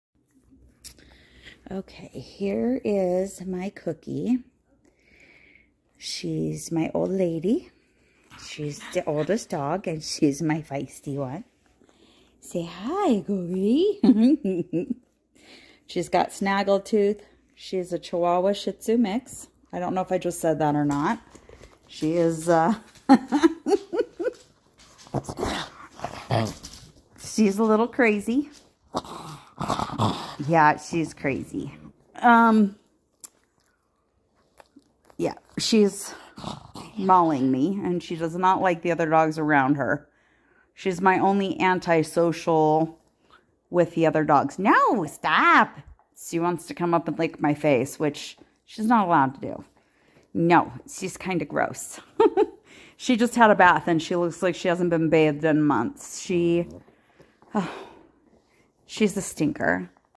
4 of 4 of the fluffy crew. The crazy dog
This is cookie she is a Shih-tzu/Chihuahua mix.  She looks shih-tzu, acts 100% Fiesta Chihuahua.  Honestly, she's the sweetest.  well actually they are all sweet,  but when it comes to people she LOVES PEOPLE.  Oh the stories I have about her. we've had her since she was a puppy, she was kept in a laundry basket her whole life before us and had to learn to walk.  she's been going 100mph since.  People still think she's a puppy, but she's 9.